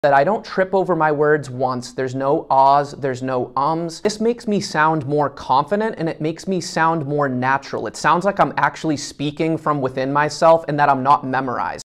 BE CONCISE on Cold Calls
Being concise is a very important skill to sounding confident and mastering cold calling.